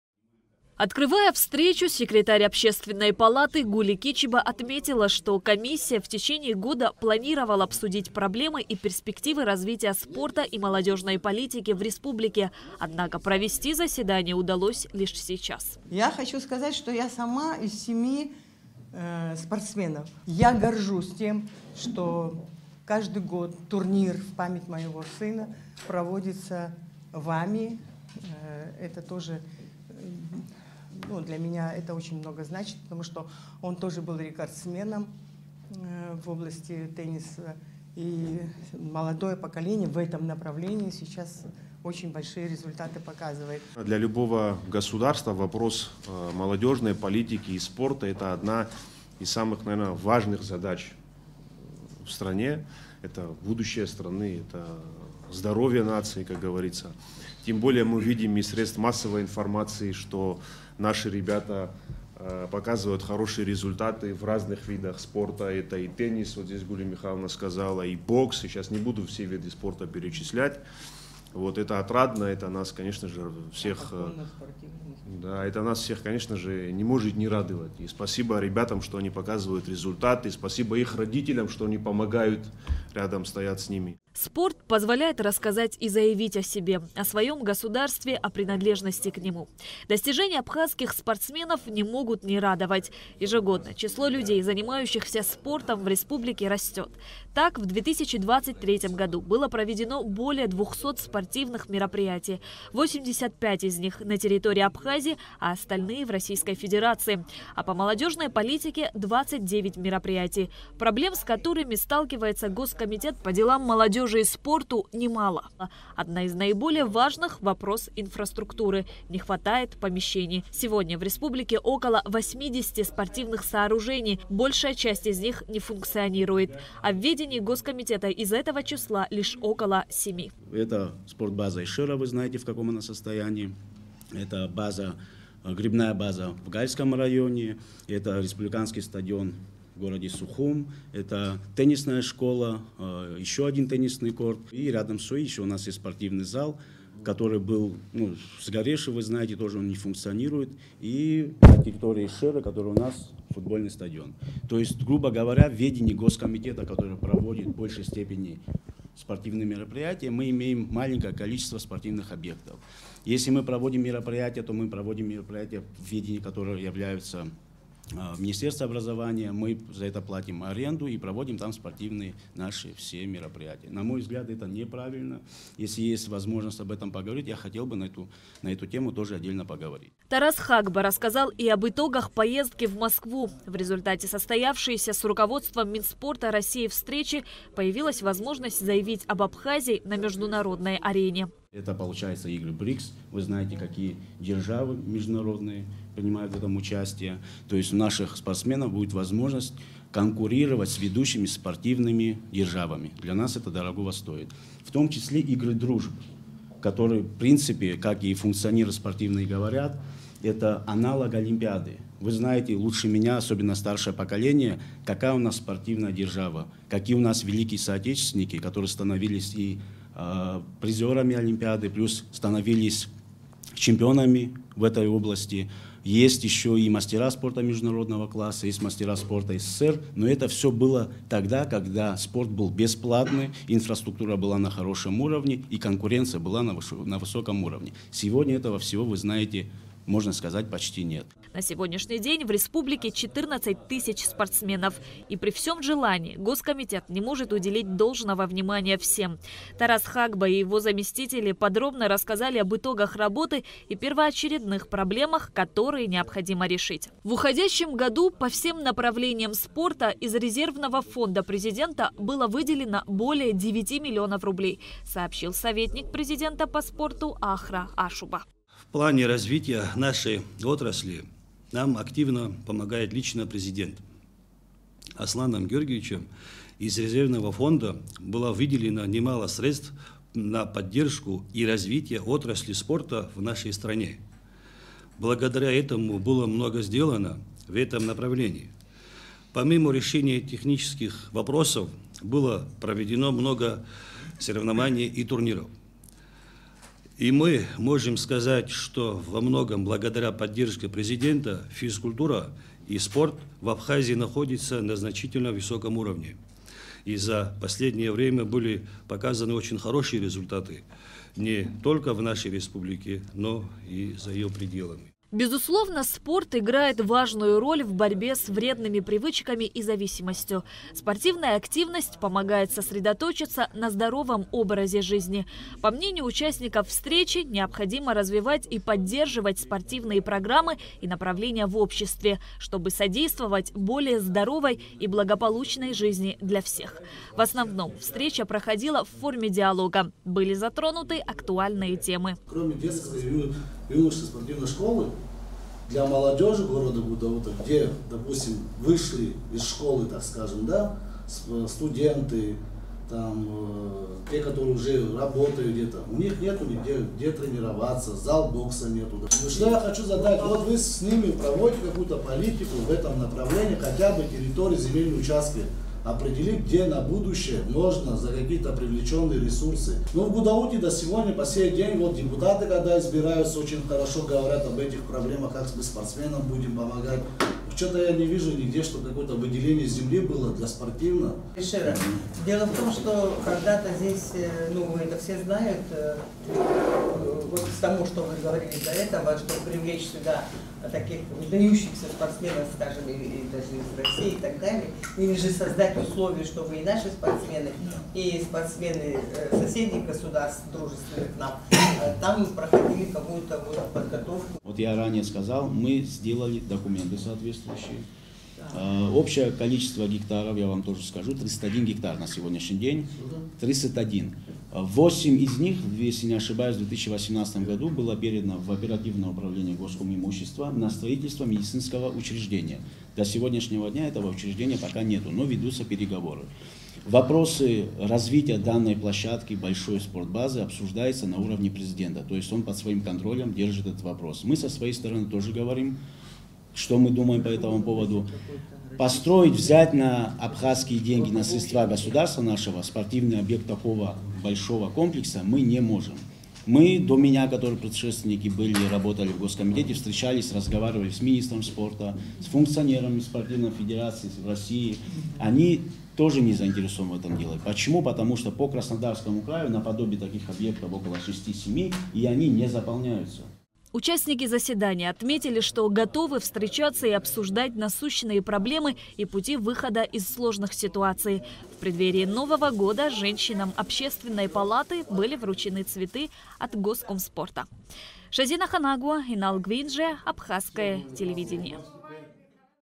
В ОП прошло расширенное заседание комиссии по здравоохранению, спорту и делам молодежи
В Общественной палате прошло расширенное заседание комиссии по здравоохранению, спорту и делам молодежи. Участники обсудили проблемы и перспективы развития спорта и молодежной политики в Абхазии.  Во встрече приняли участие советник президента по спорту Ахра Ашуба, председатель госкомитета по делам молодёжи и спорту Тарас Хагба и его заместители. Репортаж Шазины Ханагуа.